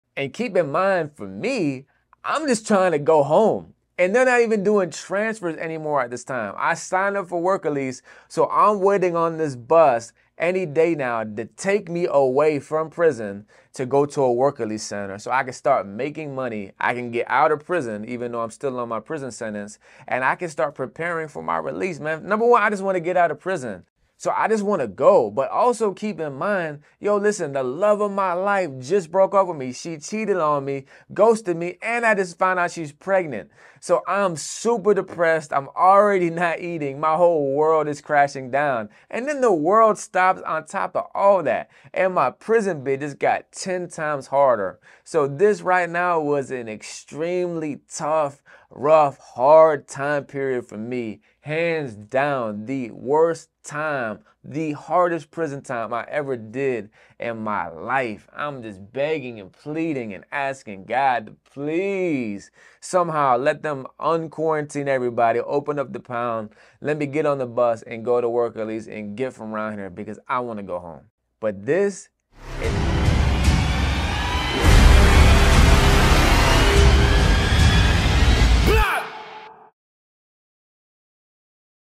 Will I Make It Out Of Prison?
If you were in prison, and had a release date, you would just want to make it home. Here E.i. the King talks about just wanting to get out of prison, and make it home alive...